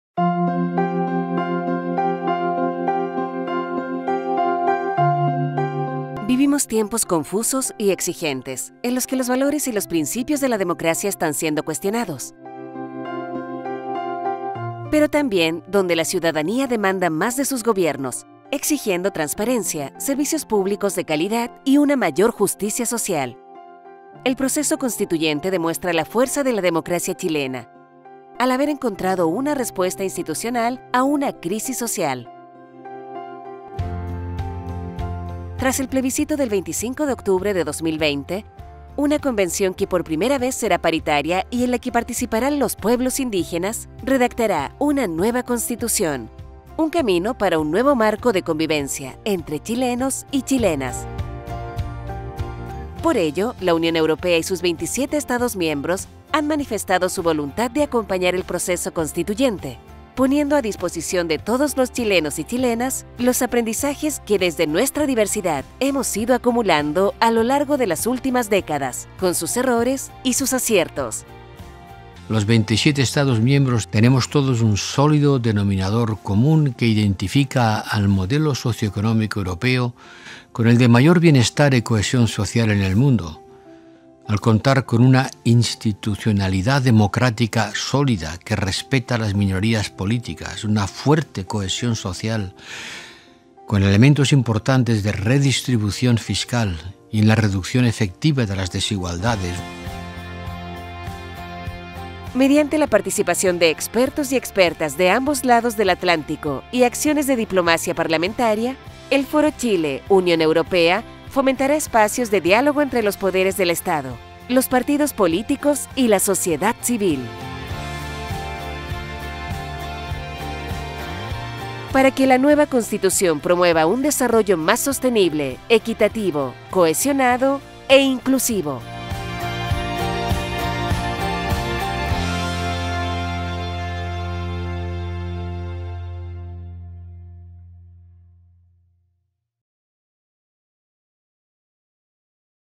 Foro Chile-Unión Europea. Proceso constituyente (español)
El proceso constituyente en marcha en Chile es uno de los más innovadores de los últimos tiempos. La elección de la Convención Constitucional tiene una composición paritaria e incorpora la participación de los pueblos originarios situando dicho proceso en la vanguardia del Derecho constitucional comparado. En este contexto, la mirada más allá de las propias fronteras es una herramienta valiosa para nutrir esta reflexión. Es por ello que la Delegación de la Unión Europea en Chile y la Agencia Chilena de Cooperación Internacional-AGCID, con el apoyo del Programa EUROsociAL+, han puesto en marcha la iniciativa “Foro Chile-Unión Europea” con la que se pretende intercambiar experiencias tanto sobre el proceso constituyente como sobre contenidos esenciales como la cohesión social, el acceso a un sistema de bienestar o la garantía de los derechos fundamentales, mostrando los aprendizajes de las Constituciones europeas desde su diversidad.

Esta iniciativa se ha articulado en torno a tres ejes:

1) Acompañamiento al debate con actores políticos en torno al ciclo de seminarios web "Diálogos Chile-Unión Europea", junto con la Cámara de Diputadas y Diputados y la Biblioteca del Congreso Nacional de Chile. Estructurado en torno a seis Mesas de Diálogo (15 sesiones) cada una de las cuales abordará distintas temáticas, y tendrán lugar entre enero y junio, hasta la instalación de la Convención Constitucional.

2) Contribución a la participación ciudadana en el debate público en torno al Ciclo de debates “Contrapuntos UE-Chile" para el proceso constituyente”, juntamente con la Universidad de Chile, la Universidad de Santiago de Chile y la Universidad de Concepción y está dirigido a promover el debate y la participación ciudadana. De junio a octubre de 2021.

3) Actividades específicas con instituciones nacionales desde donde se abordan distintas acciones con instituciones gubernamentales chilenas en torno a temas de interés común.